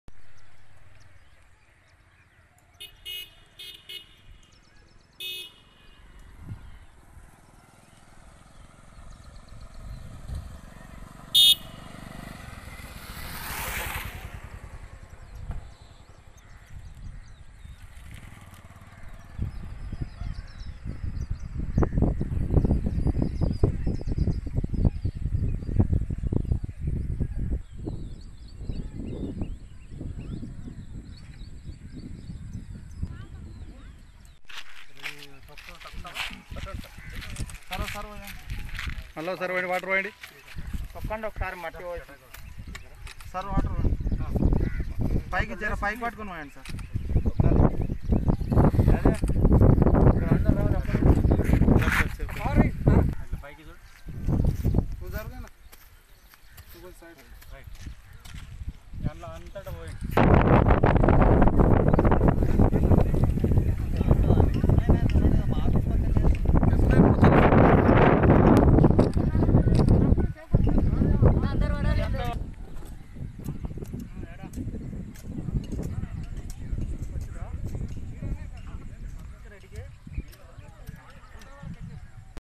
S9 NEWS MBNR హన్వాడ మండలం వేపూర్ గ్రామంలో ఈత మొక్కల నాటిన ఎక్సైజ్ ఎస్ఐ మనోహర్
UMMDI MAHABUBNAGAR -OUR DISTRICT OUR NEWS